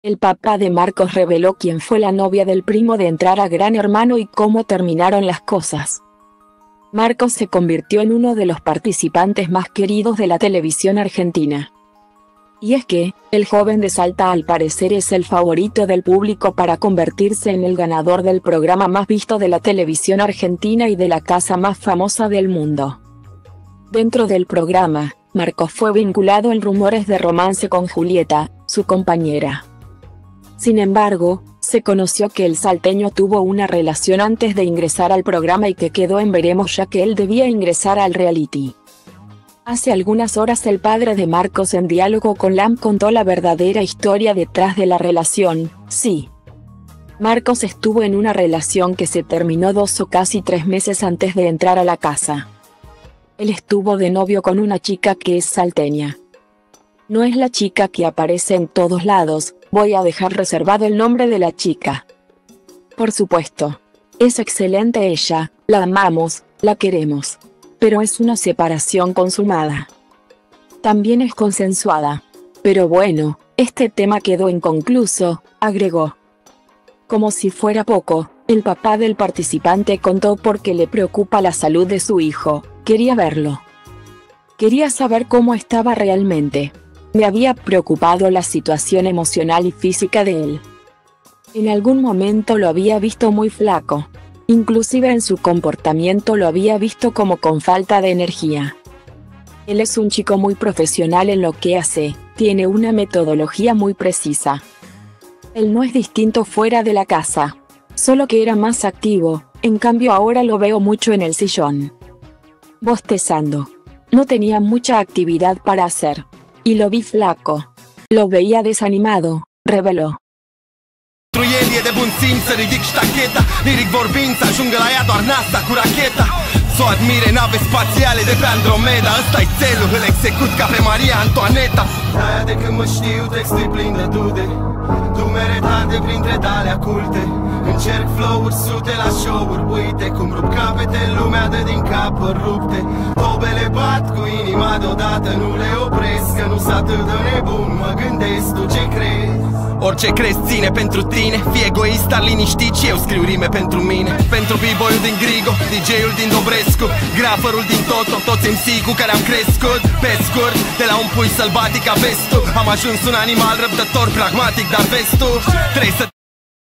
El papá de Marcos reveló quién fue la novia del Primo de entrar a Gran Hermano y cómo terminaron la
El papá de Marcos reveló quién fue la novia del Primo  de entrar a Gran Hermano y cómo terminaron las cosas
Marcos se convirtió en uno de los participantes más queridos de la televisión argentina.Y es que, el joven de Salta al parecer es el favorito del público para convertirse en el ganador del programa más visto de la televisión argentina y de la casa más famosa del mundo. Dentro del programa, Marcos fue vinculado en rumores de romance con Julieta, su compañera.